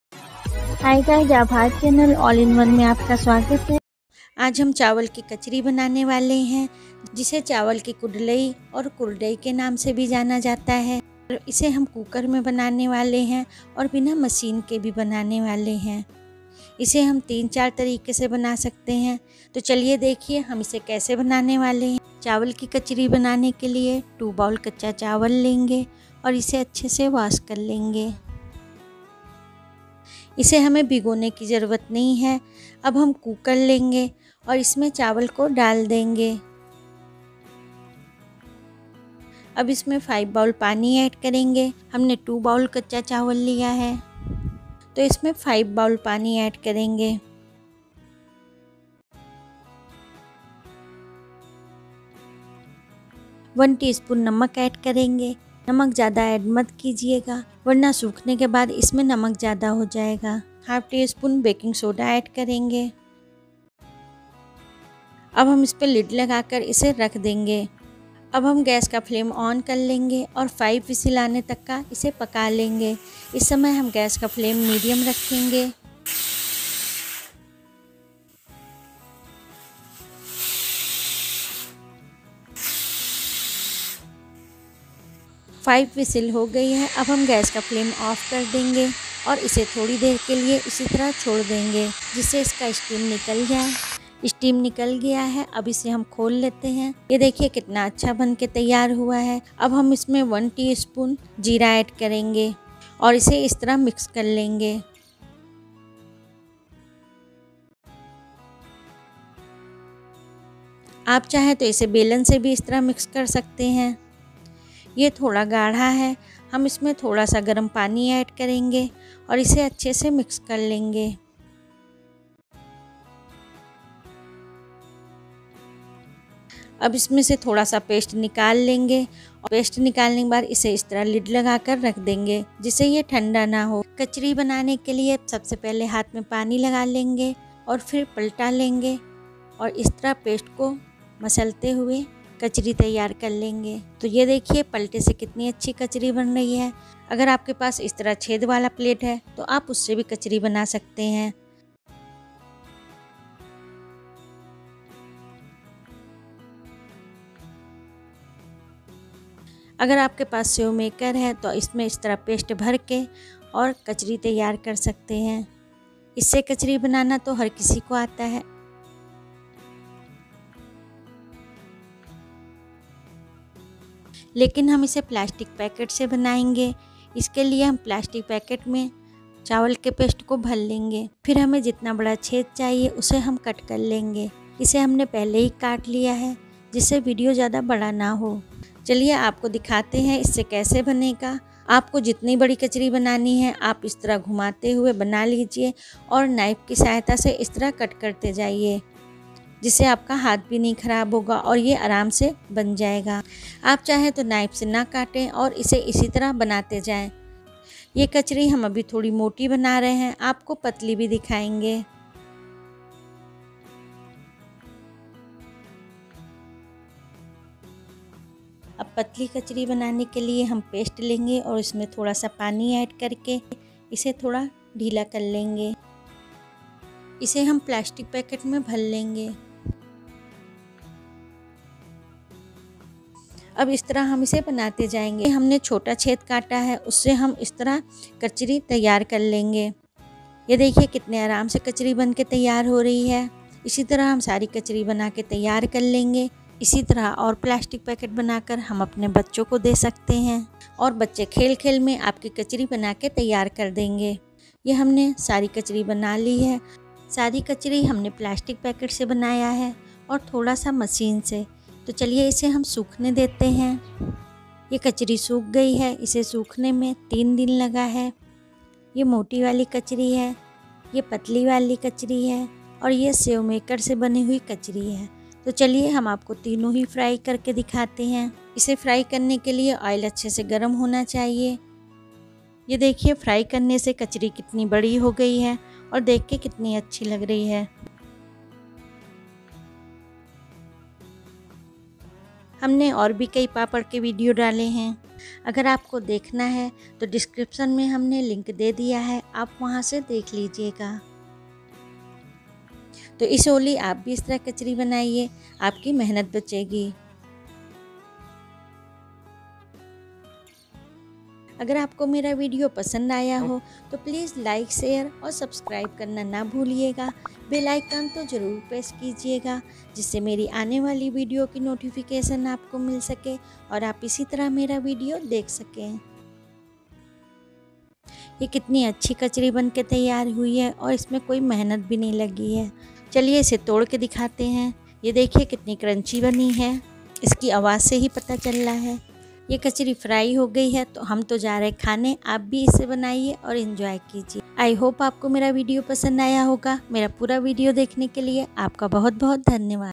बिना मशीन कुकर मे बनाए चावल की कुडलई (कुरडई ) | Chawal ki kachari kaise banaye| Chawal ke papad
Is video me bataya gya hai ki Chawal se kachari kaise banate hai
कुकर मे बनाए चावल की कुडलई (कुरडई ) | Chawal ki kachari kaise banaye| Chawal ke papad | चावल